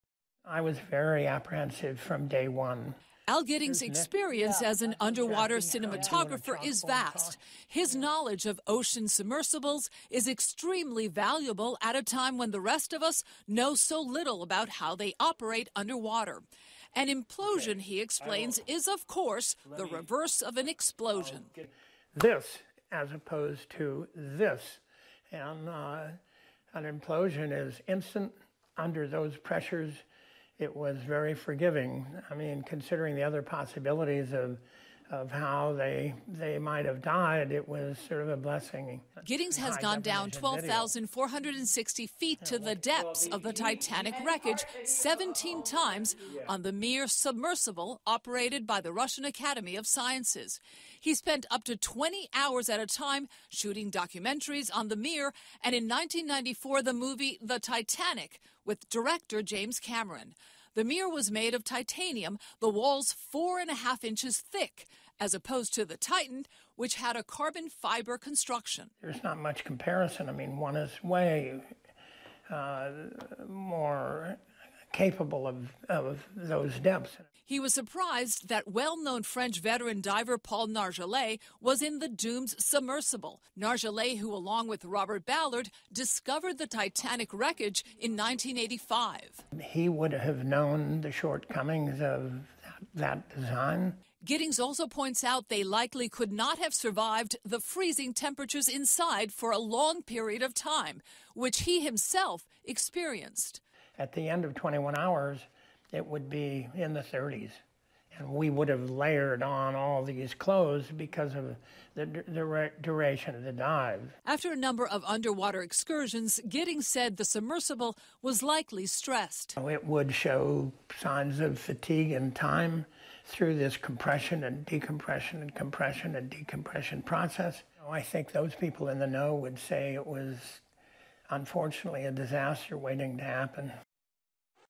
Titan sub tragedy was 'disaster waiting to happen': expert
Experienced underwater cinematographer Al Giddings who also worked on the film "Titanic," says the sub excursion was a "disaster waiting to happen."